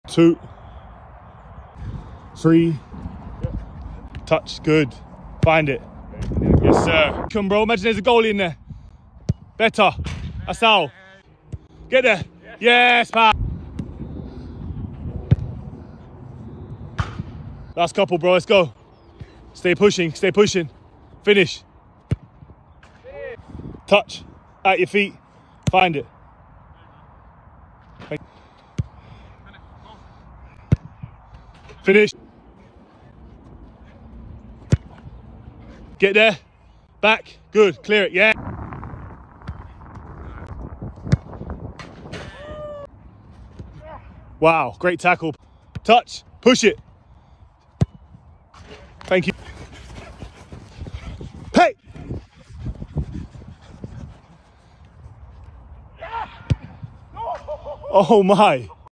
Passing, shooting and defensive football session with Taylor Richards, Jon Panzo & Christian Saydee
Passing, shooting and defensive football session with Brighton’s Taylor Richards, Nottingham Forest’s Jonathan Panzo & Brentford’s Christian Saydee